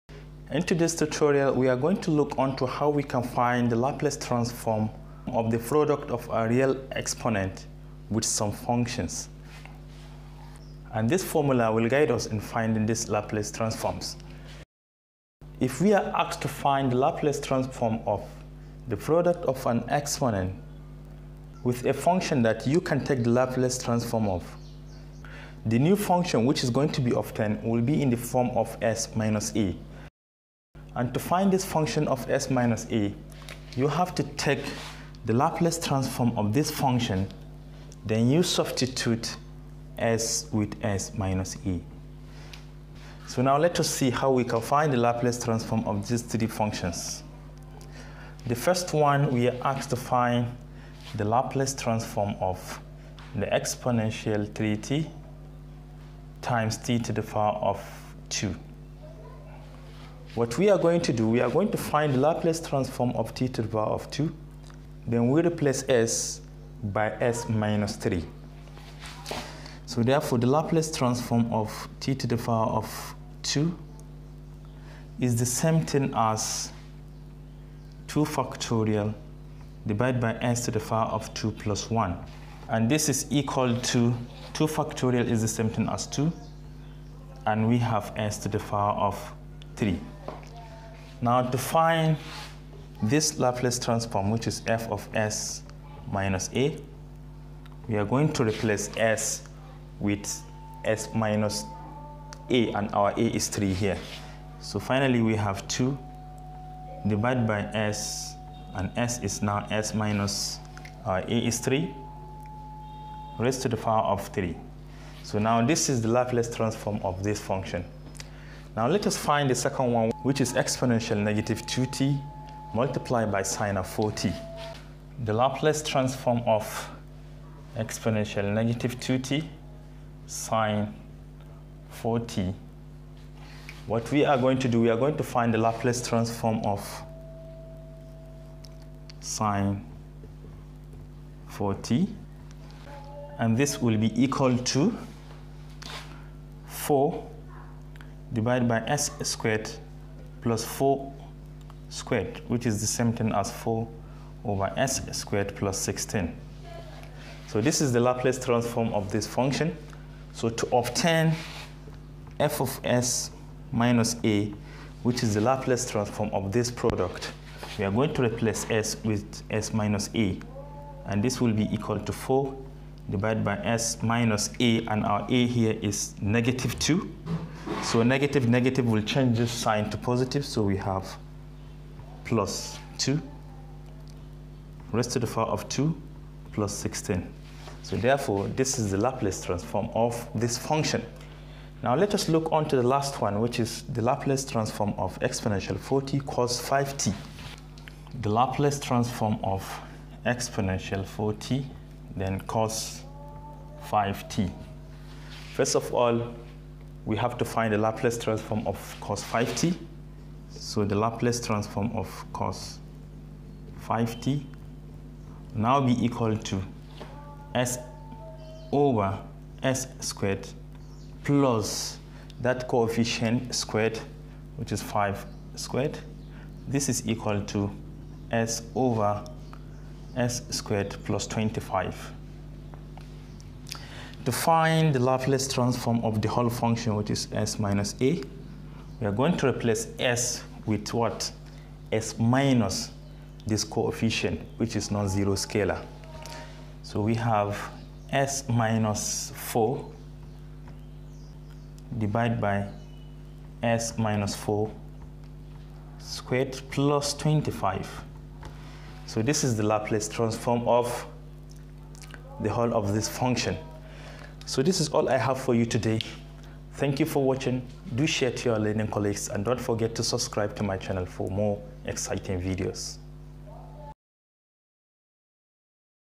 Laplace Transform | Shifting theorem
The first shifting theorem provides a convenient way of calculating the Laplace transform of functions that are of the form
f(t) := e-atg(t)
where a is a constant and g is a given function.

ζ{e-atg(t)} = F(s-a)
To obtain F(s-a), you need to take the laplace transform of g(x) then you replace s with (s-a) to obtain the laplace transform of ζ{e-atg(t)}